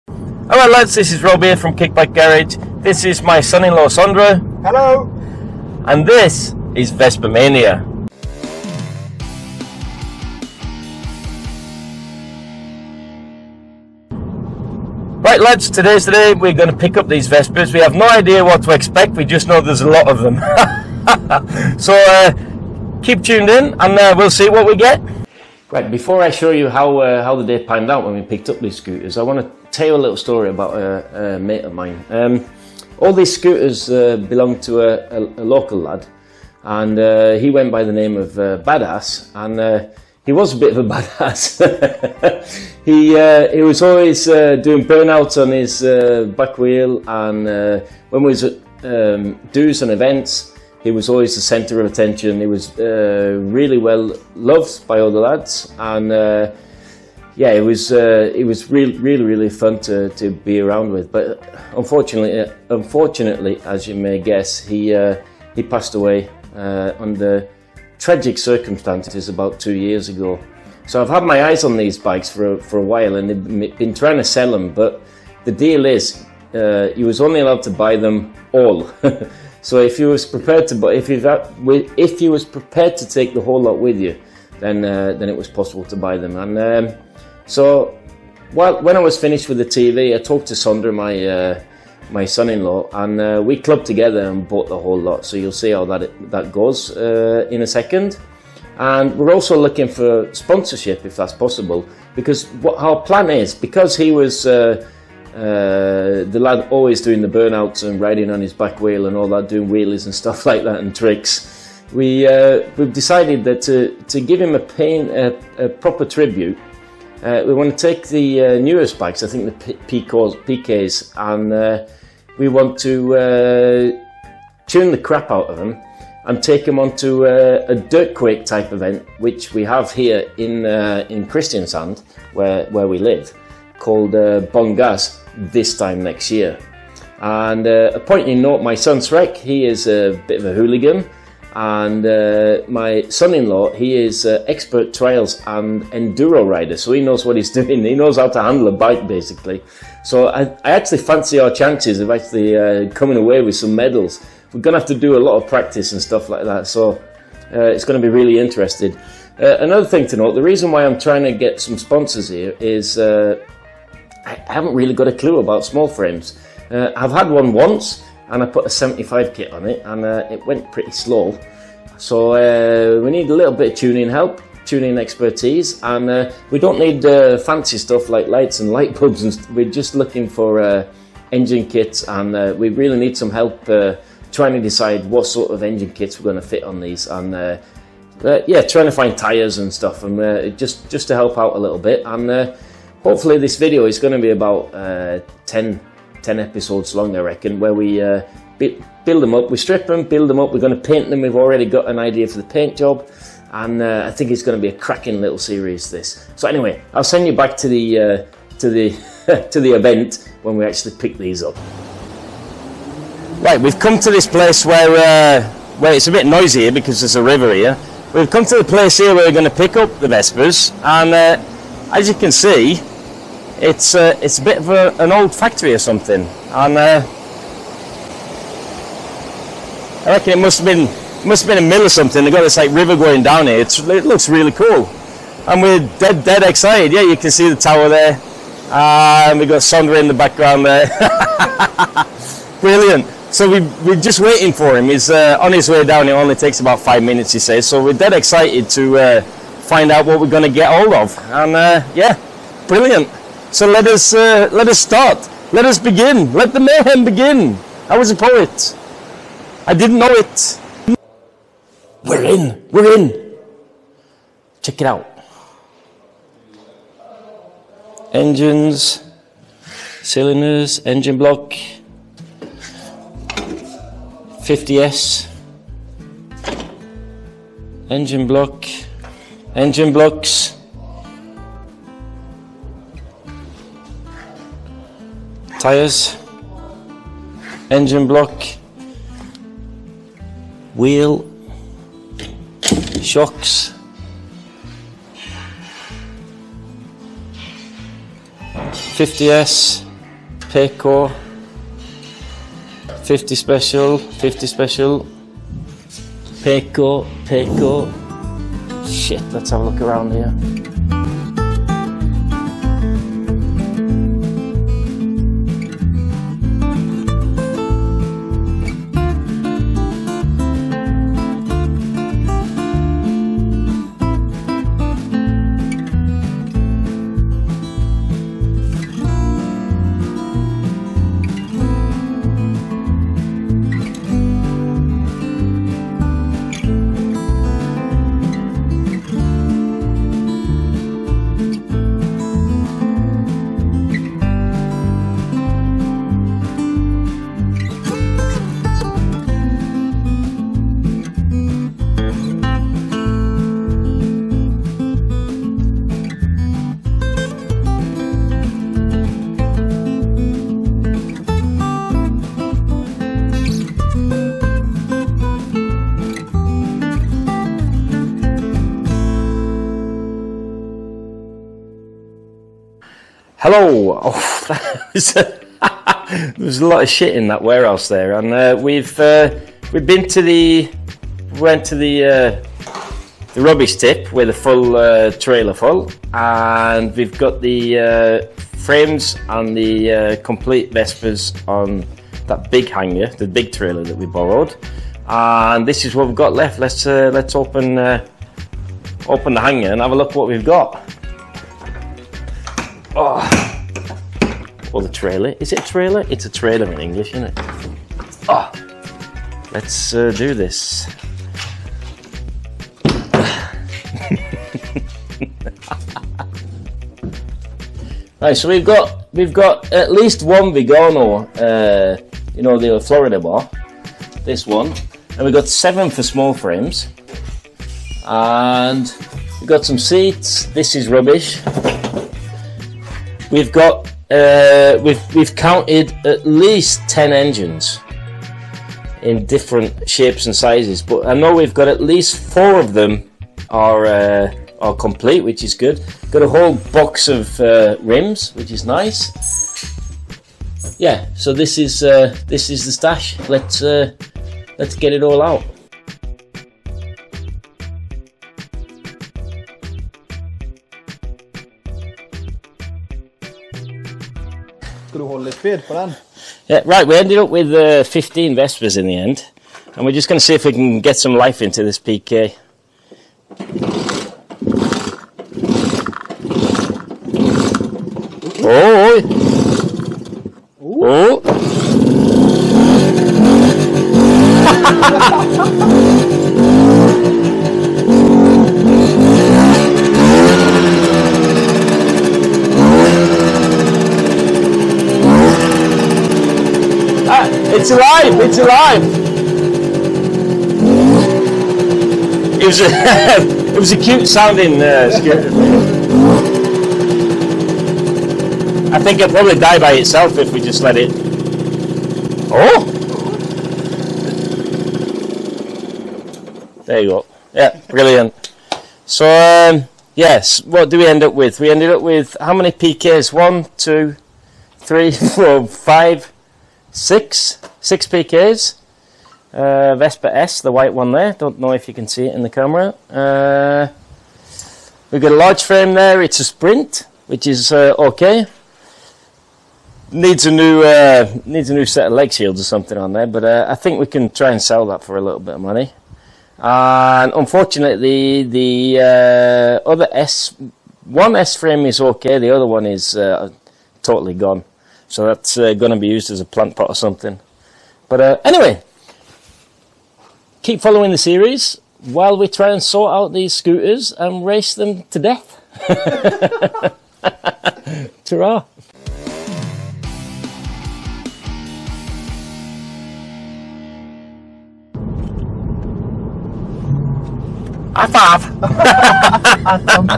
Vespamania Part 1
This is the start of out journey to build up and race vespas on the dirt oval!